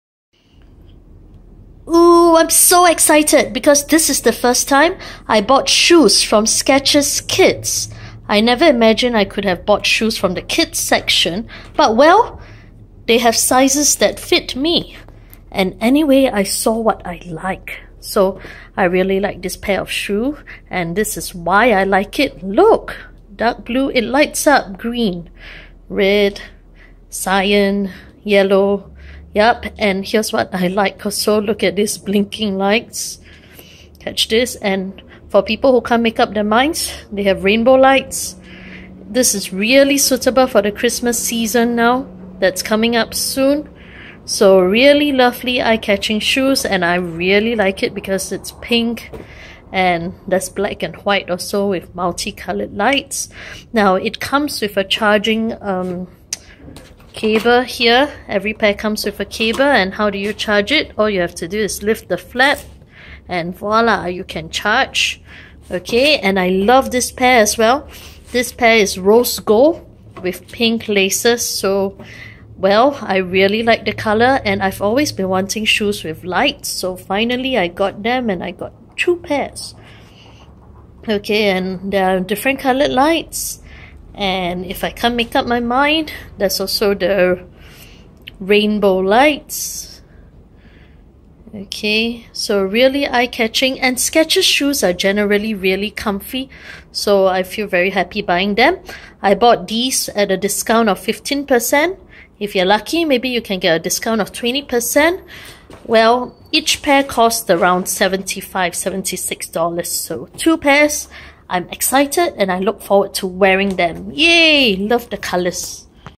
Skechers Shoes
First time I bought shoes from the kids' section and I'm excited!! =) I love the colours and the blinking lights!!! Fun!! =) Looking forward to wearing them for the upcoming Christmas season!! =)